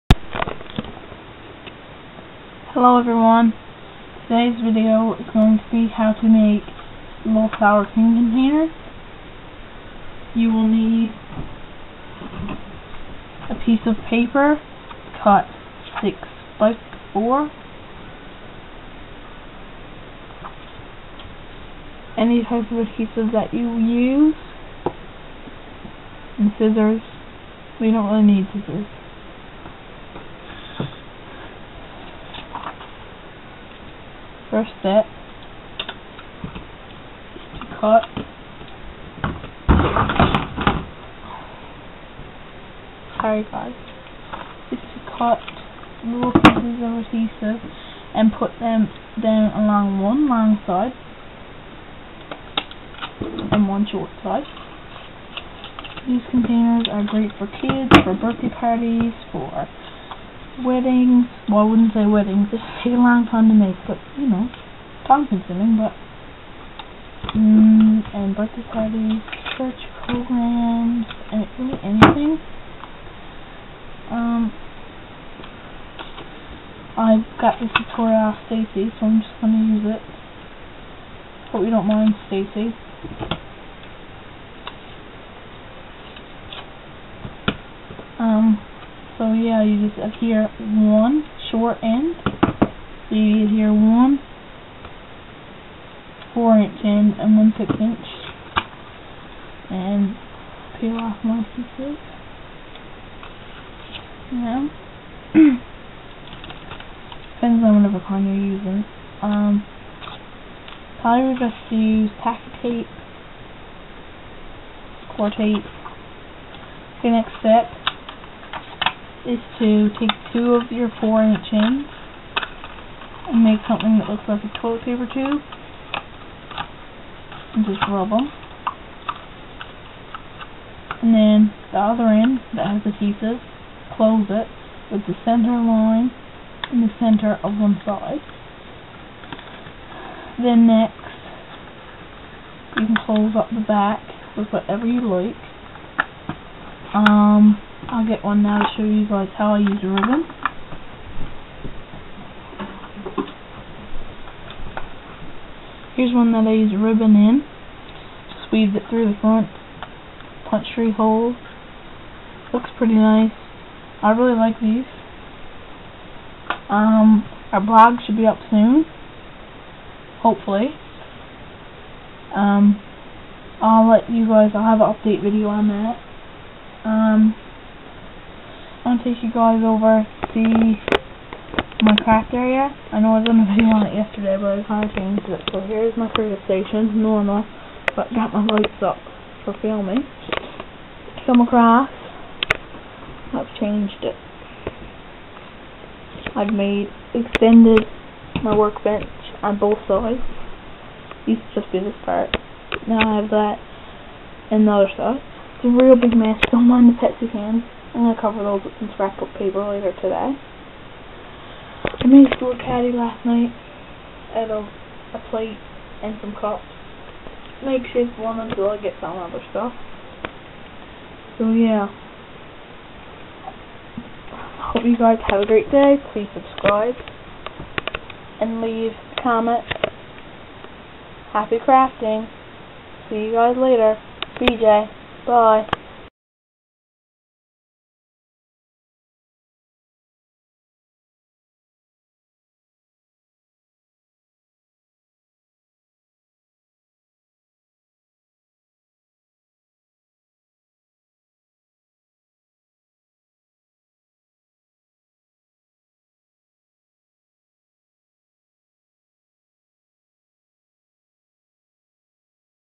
Tuesday Tutorial: Party Favor containers (sour cream containers)
Hey Everyone... Hope you enjoy.. If you have any ideas about how i could inprove my video quality please share them! Thanks to Stacy for the Idea! Blog will soon be up (I hope)